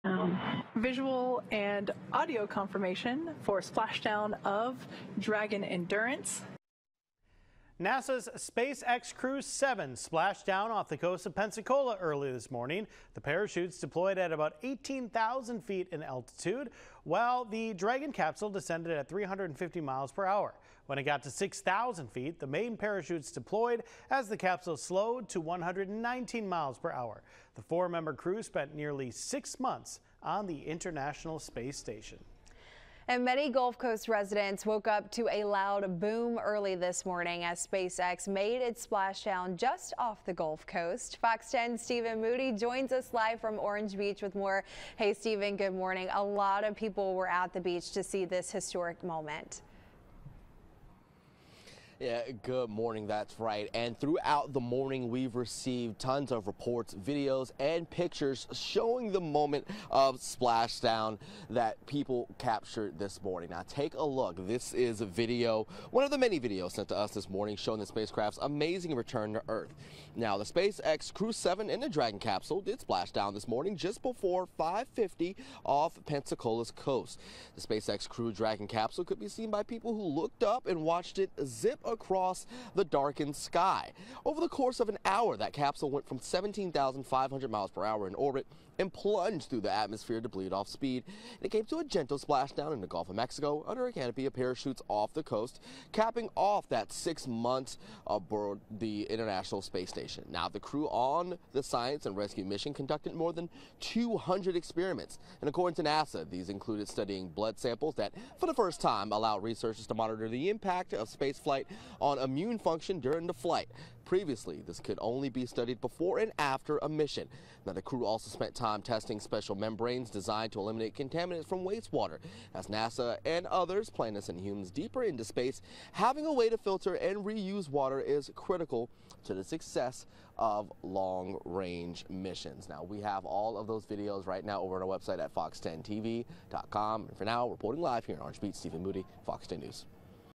SpaceX Dragon Endurance spacecraft splashes down off Pensacola coast
SpaceX's spacecraft returned the NASA Crew-7 mission after nearly six months in orbit.